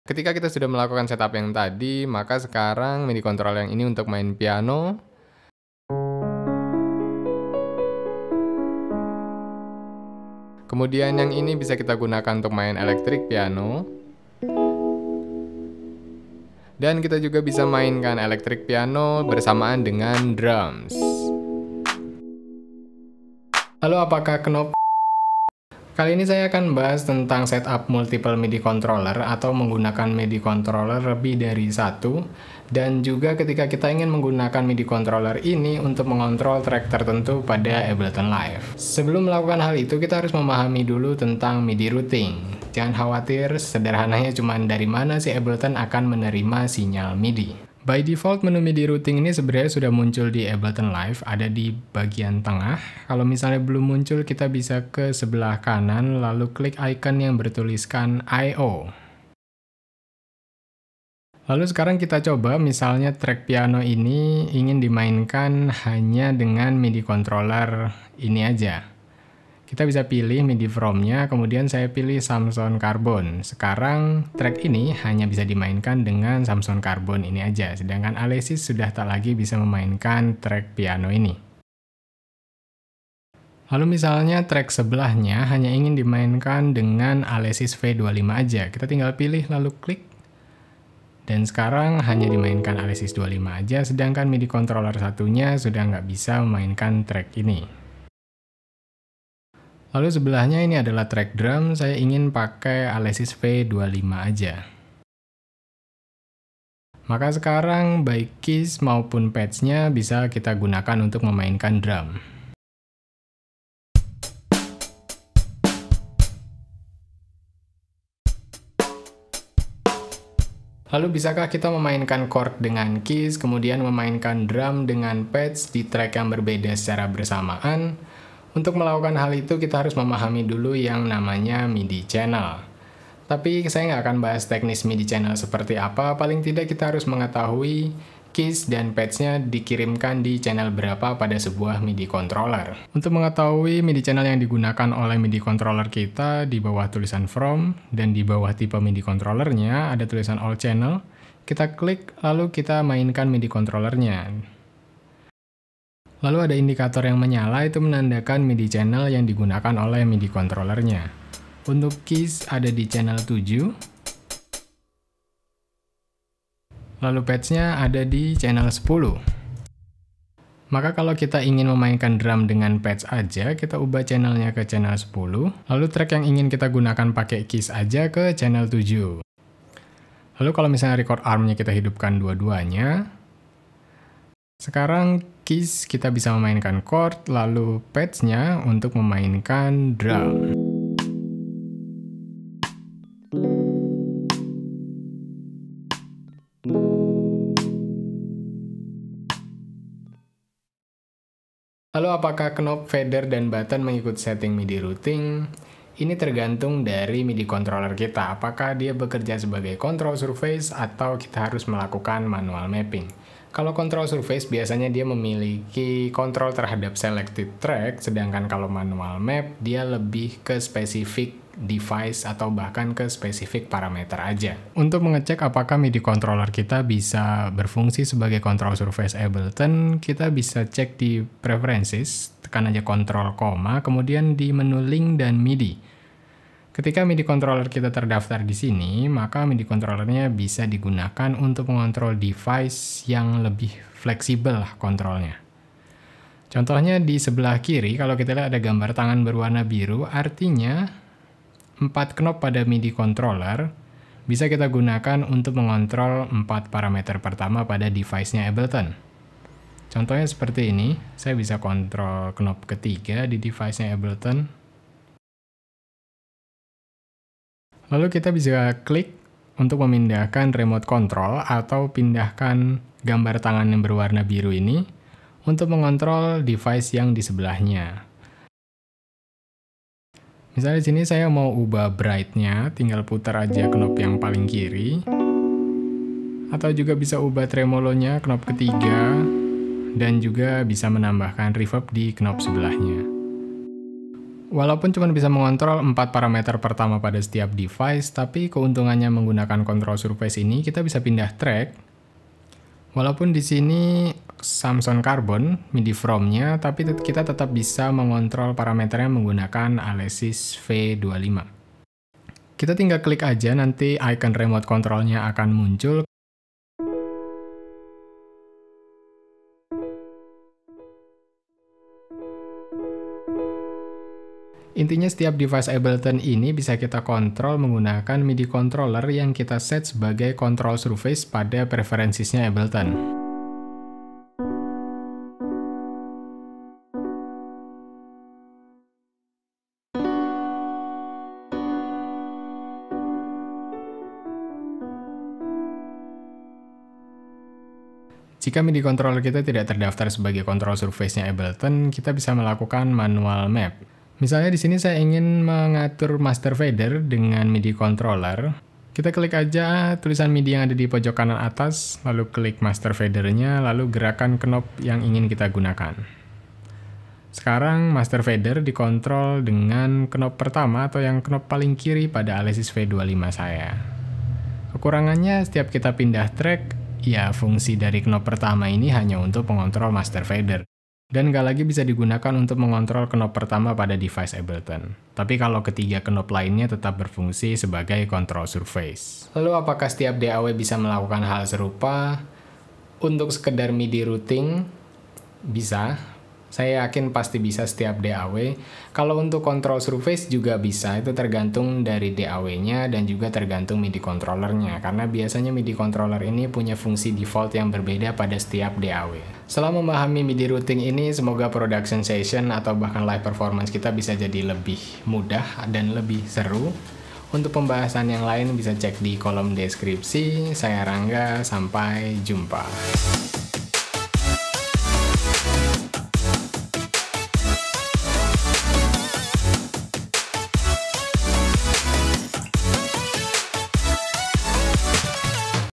Setup Multiple Midi Controller | Ableton Live Tutorial
Divideo ini akan membahas bagaimana setup multiple midi controller pada ableton live, mungkin kita menginginkan midi controller hanya dipakai untuk mengontrol sebuah track secara spesifik entah untuk kebutuhan live performance atau hanya sekedar untuk membuat sesi produksi musik jadi lebih asik.

Bagaimana cara menggunakan lebih dari satu midi controller ?
Bagaimana cara menggunakan knob pada midi controller ?

Disini saya menggunakan Midi Controller Alesis V25 & Samson Carbon 49

Silahkan klik timestamp dibawah untuk menuju ke spesifik topik yg diinginkan ~

Topik yang dibahas :
00:00 Setup Banyak Midi controller
00:41 Ableton Midi Routing
02:11 Midi Channel (independent keys & pads)
03:29 Midi Control Surface (menggunakan knob midi controller)
06:29 Apakah DAW Lain Bisa ?